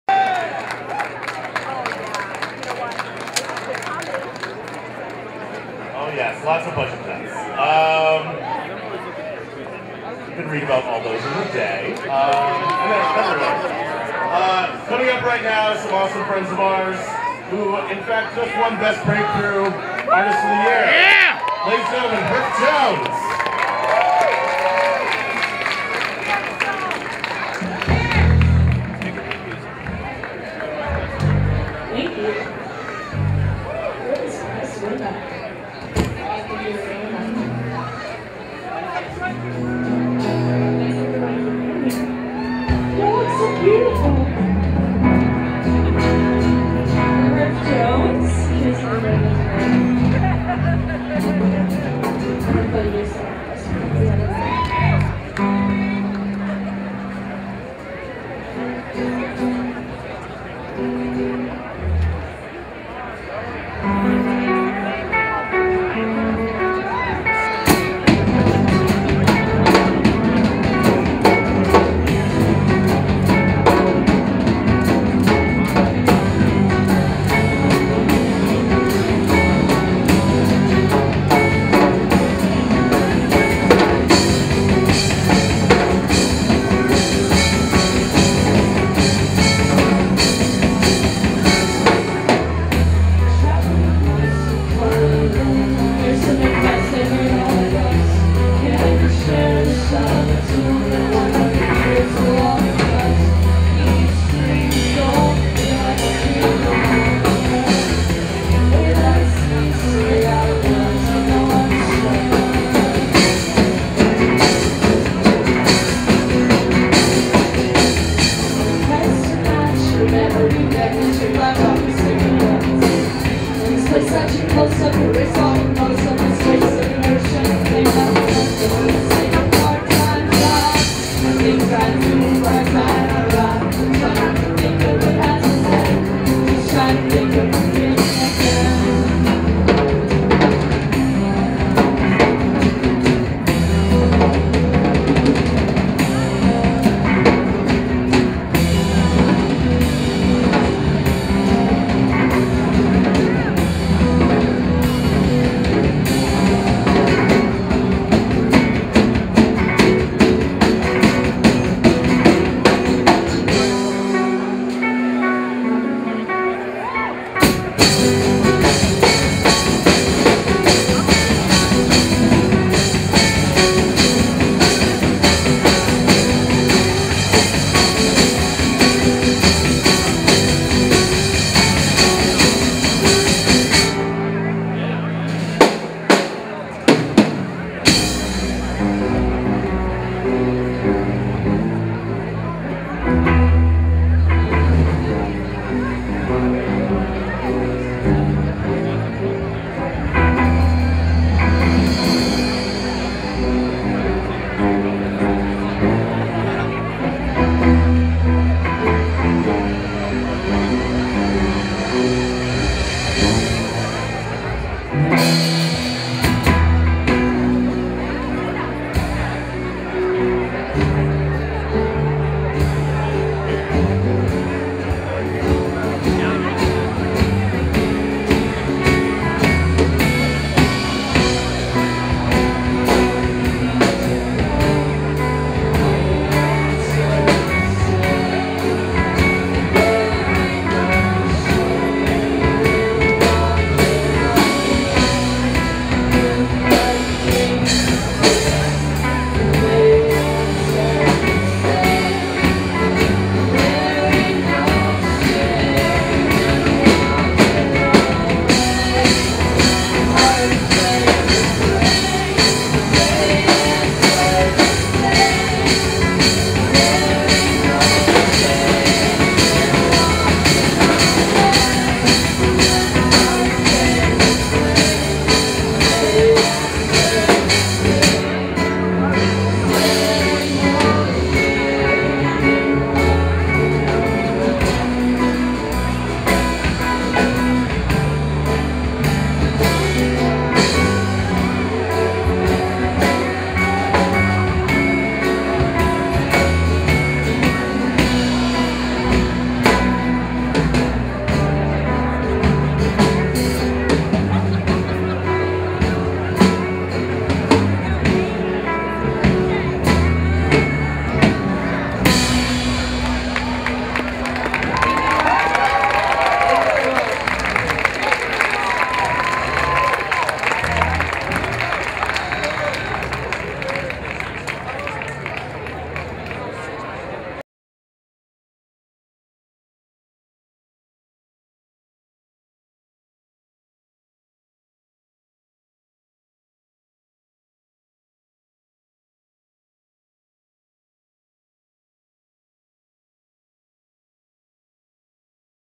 Herff Jones @ The Whalie Awards
The Whalie Awards Are New London's Music Award Show.
Produced By Sean Murray and Rich Martin

Filmed and Edited By Andrew W Proctor